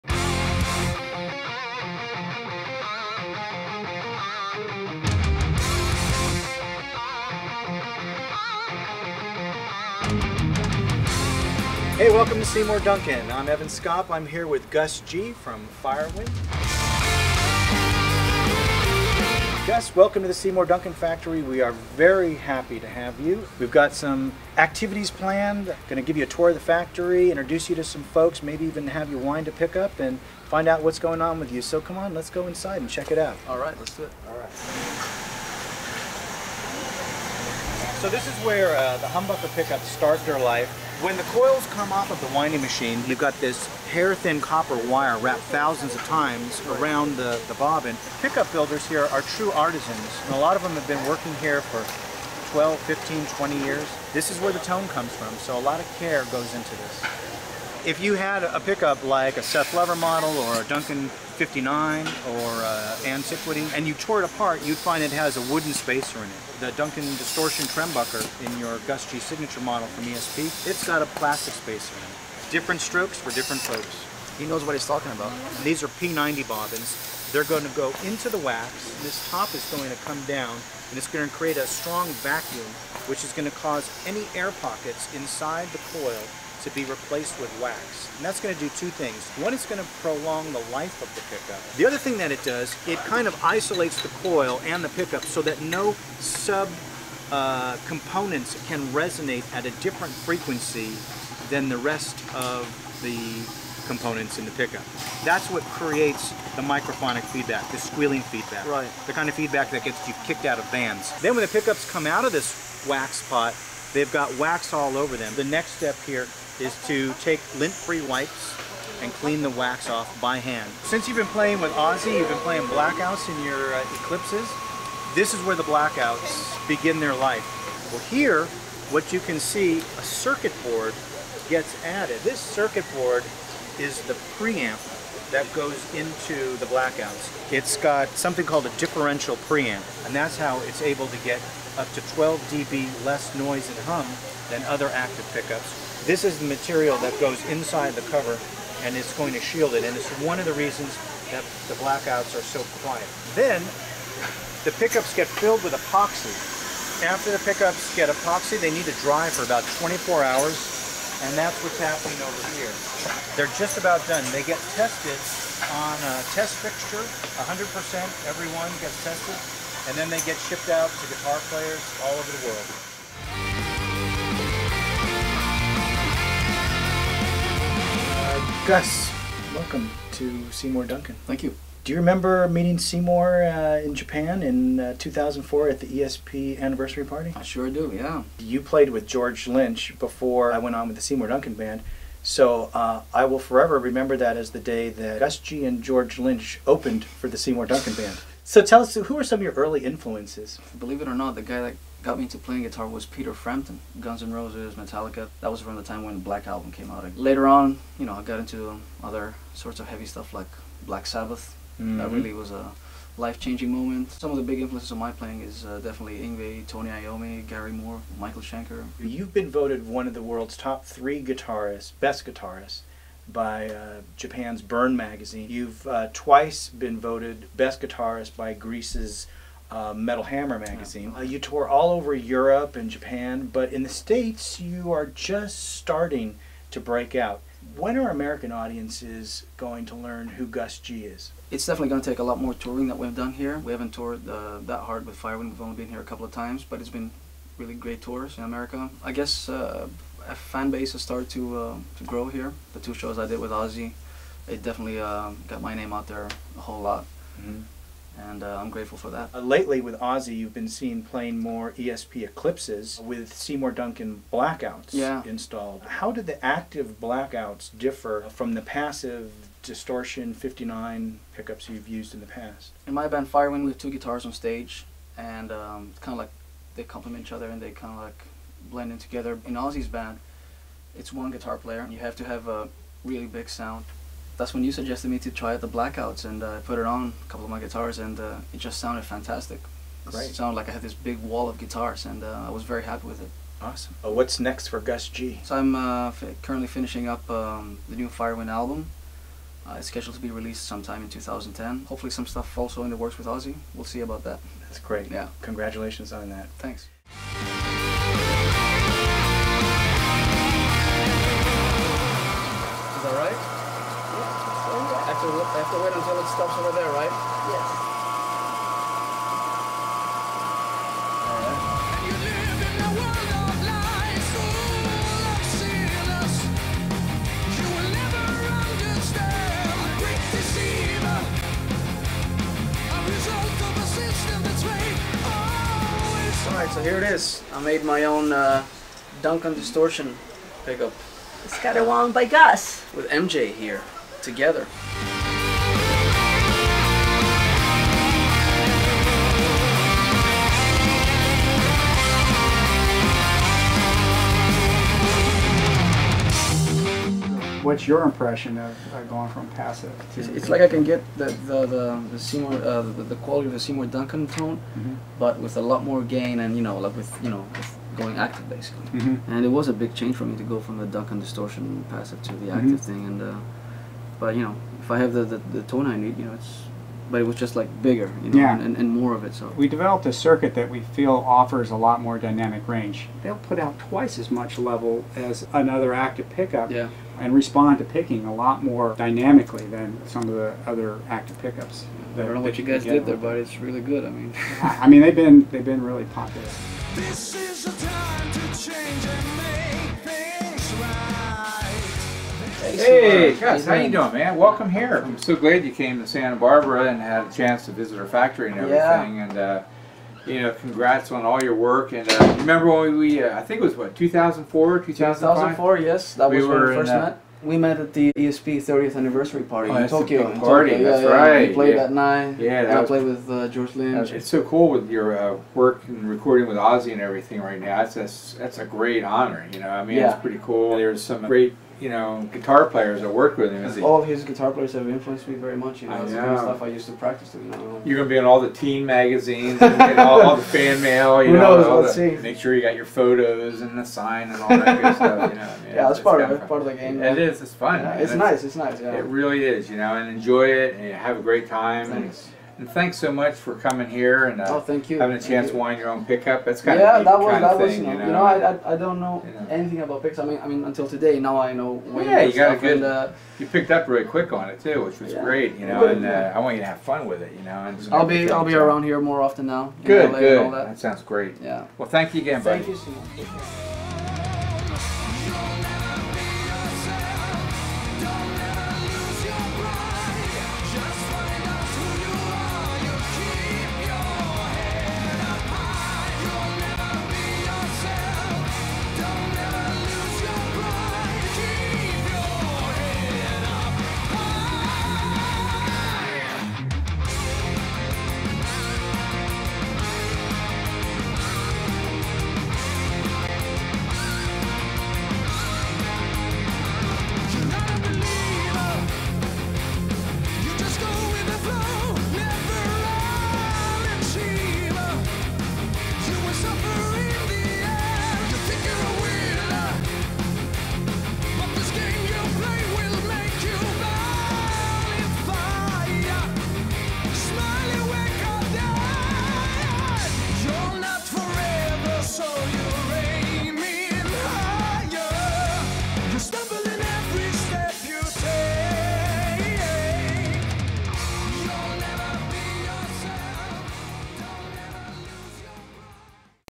Gus G Visits The Seymour Duncan Factory
Recently Gus stopped by the Seymour Duncan factory for a tour and to meet the crew. He had a chance to hang out with Seymour, wind a pickup with Maricela Juarez, discuss the unique tonality of Blackouts with their designer Kevin Beller, and talk with Evan Skopp about the future for this up and coming guitar virtuoso.